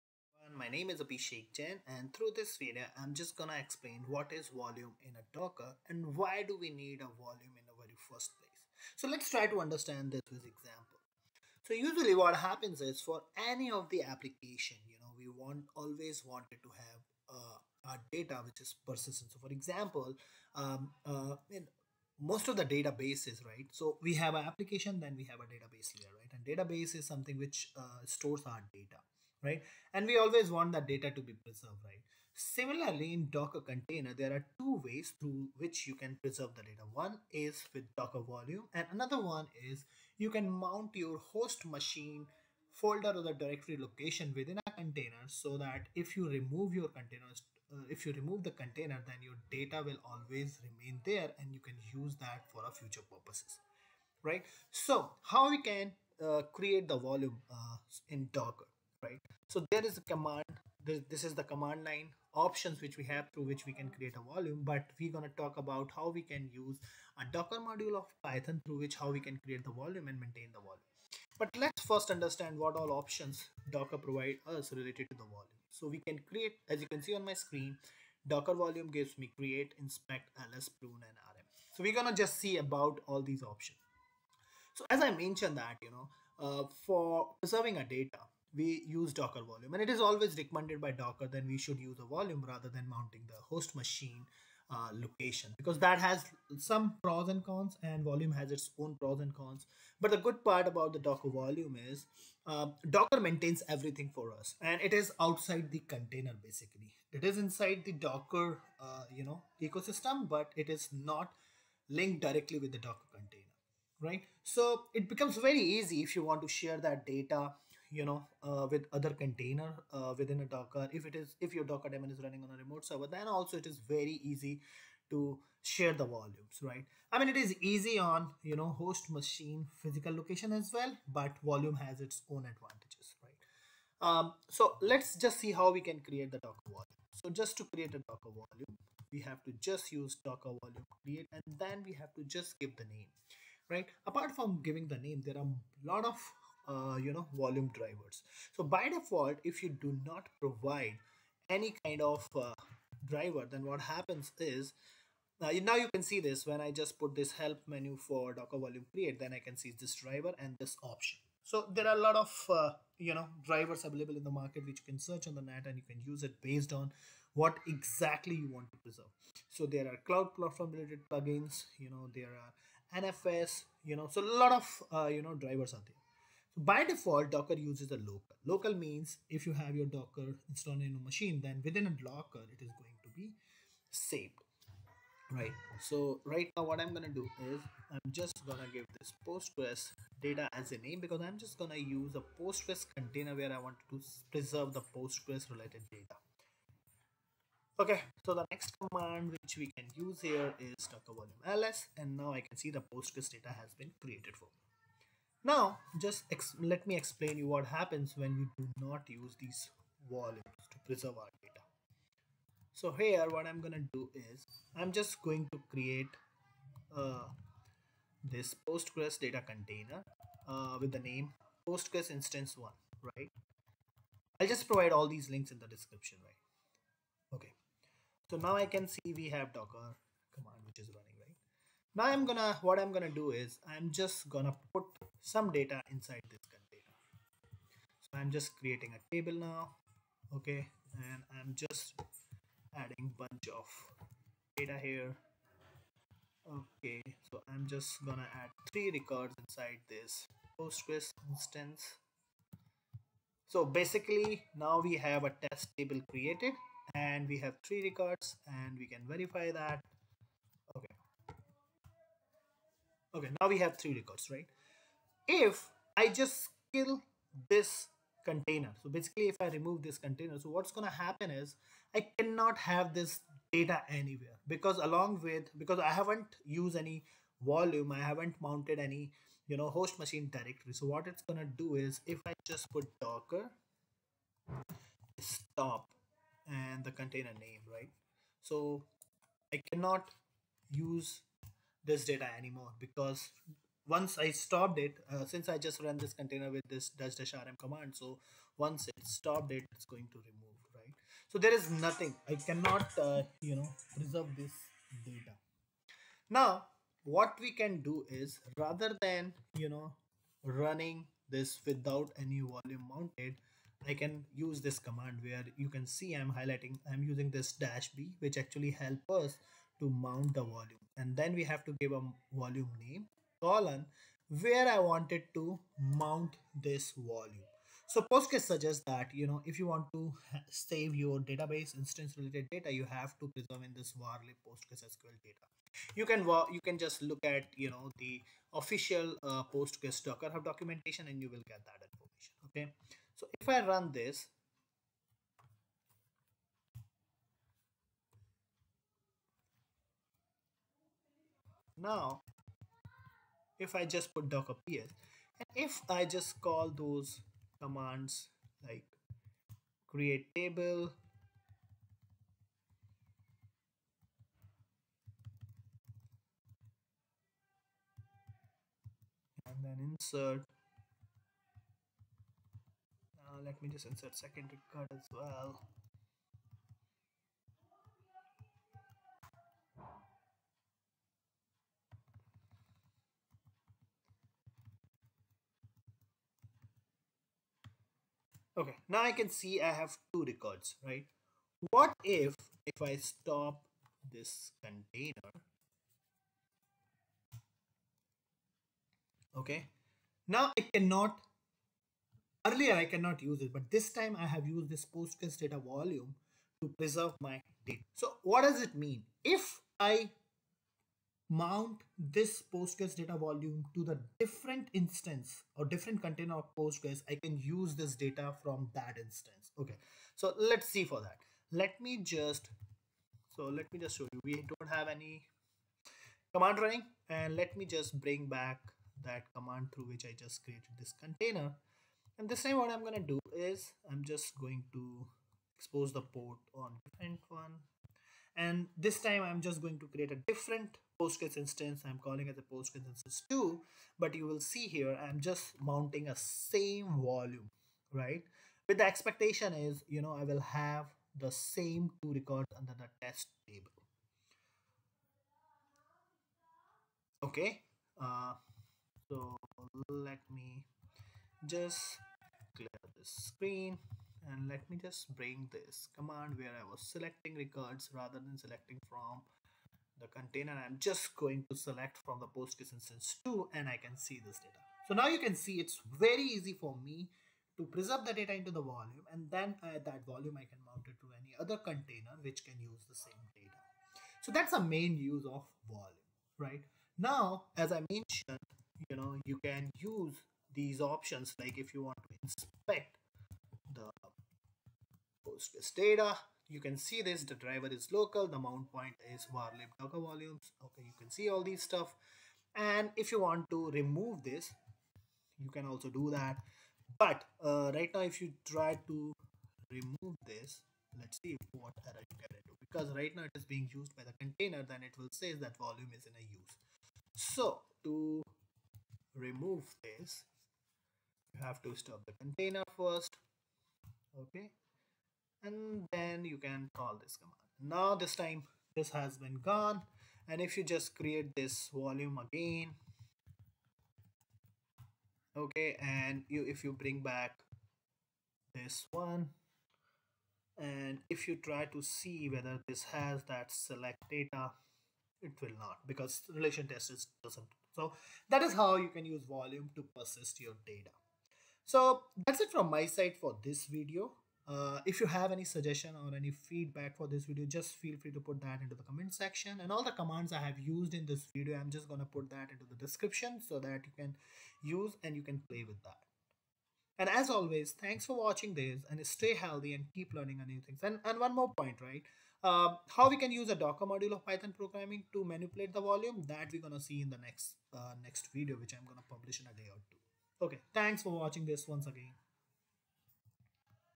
Python-Docker (What is Volume & How to create/persist container data with volume) - Part4
This part explains

1. What is volume in docker
2. How to persist data in docker container with volume
3. How to list/remove volumes
4. How to share persist data across containers
5. How to preserve postgres data with volumes

Used Commands -

To create volume -
docker volume create postgresdata

Run container with Volume
docker run --rm --name postgres-instance-1 -e POSTGRES_PASSWORD=postgres -d -p 5432:5432 -v postgresdata:/var/lib/postgresql/data  postgres
docker run --rm --name postgres-instance-2 -e POSTGRES_PASSWORD=postgres -d -p 5433:5432 -v postgresdata:/var/lib/postgresql/data  postgres

Stop Container
docker stop postgres-instance-1
docker stop postgres-instance-2

To create table/Insert Record/Select Record
docker exec -i postgres-instance-1 psql -U postgres -c "CREATE TABLE test (ID INT PRIMARY KEY, DATA TEXT);"
docker exec -i postgres-instance-1 psql -U postgres -c "INSERT INTO test VALUES (0, 'AbhishekJain');"
docker exec -i postgres-instance-1 psql -U postgres -c "INSERT INTO test VALUES (1, 'AbhishekJain1');"
docker exec -i postgres-instance-1 psql -U postgres -c "INSERT INTO test VALUES (2, 'AbhishekJain2');"
docker exec -i postgres-instance-1 psql -U postgres -c "SELECT * FROM test;"